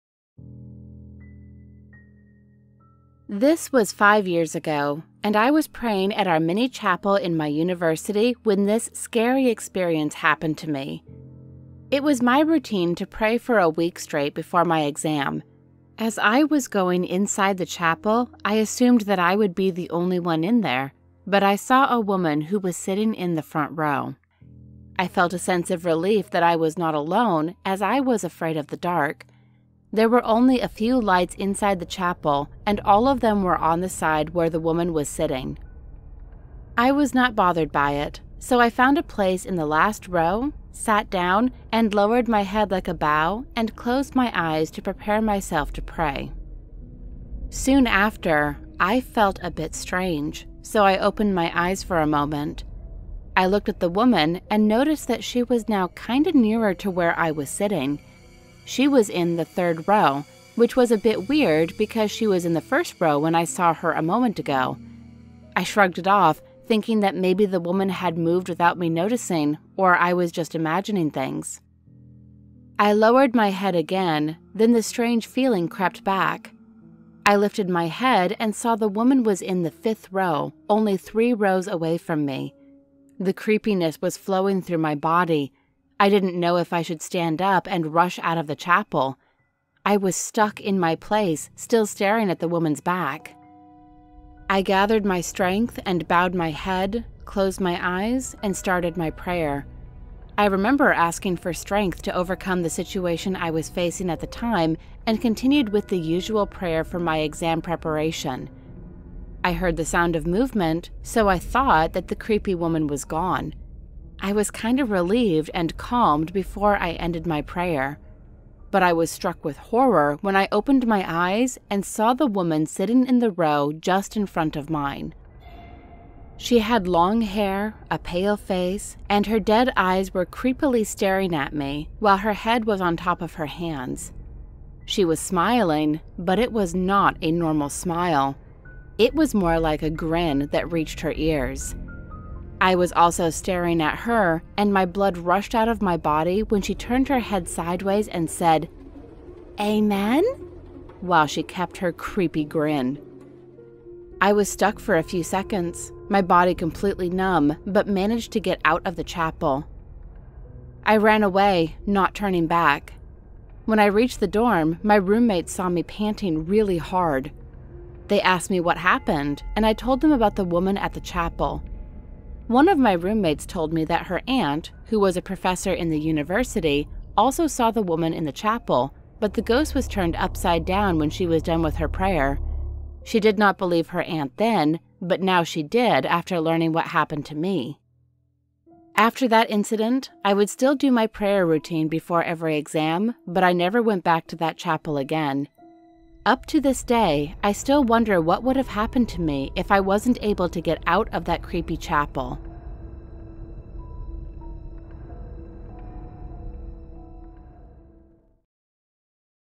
True Chapel Horror Story Animated
this is a scary &  creepy & horror animation

Animated by SUN
Story Submitted by Krizelle Trillo

Special Thanks To My Patron!!!
Chef Andre Maxwell
Jessica Page
Rifks

Special Thanks To The Wansee Members!!!
Lancerious
Joshua Perez